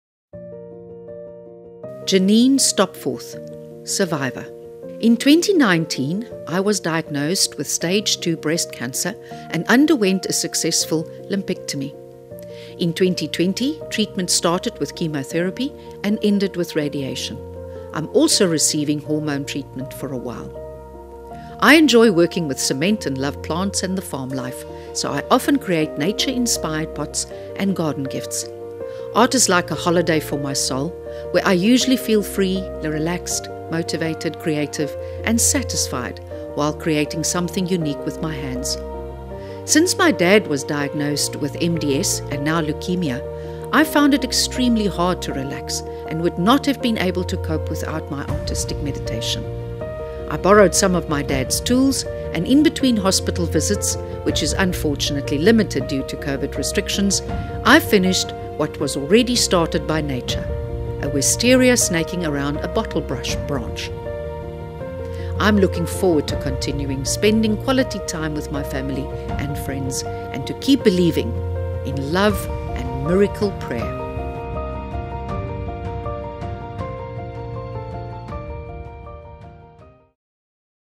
Jeannine Stopforth
In 2019, I was diagnosed with stage 2 breast cancer and underwent a successful lumpectomy.
In 2020, treatment started with chemotherapy and ended with radiation. I’m also receiving hormone treatment for a while.

I enjoy working with cement and love plants and the farm life, so I often create nature-inspired pots and garden gifts. Art is like a holiday for my soul, where I usually feel free, relaxed, motivated, creative, and satisfied whilst creating something unique with my hands.

Since my Dad was diagnosed with MDS and now leukemia, I found it extremely hard to relax and would not have been able to cope without my artistic meditation. I borrowed some of Dad’s tools and in between hospital visits, which is unfortunately limited due to Covid restrictions, I finished what was already started by nature – a Wisteria snaking around a Bottlebrush branch.

I am looking forward to continue spending quality time with my family and friends, and to keep believing in love and miracle prayer.